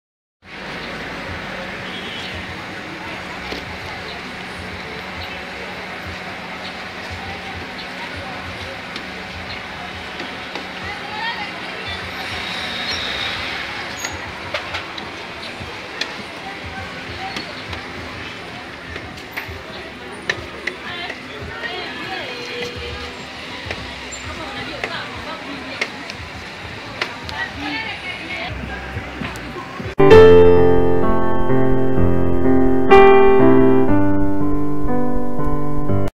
Una razón para no VOTAR al PDGE
El Mercado Central de Bata-Sunil. La realidad que la Televisión Nacional de Guinea Ecuatorial oculta a la comunidad internacional.
Todos los Pedegistas dicen ¿qué país no tiene pobres?, "tenéis que hacer criticas constructivas vosotros los opositores, sois unos envidiosos"